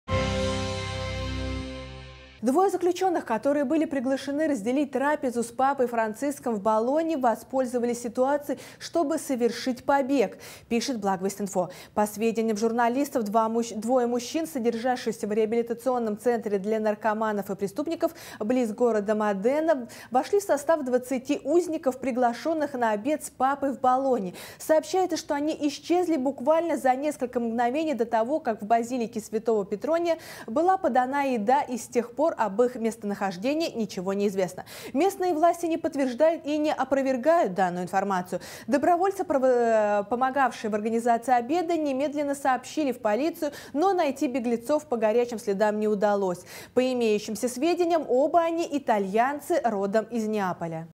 Заключенные сбежали "благодаря" понтифику
Двое из двадцати заключенных, которых пригласили на обед с Папой Франциском, бежали из под стражи. О том, кто это были о действиях добровольцев и о реакции властей на случившиеся смотрите в этой сюжете.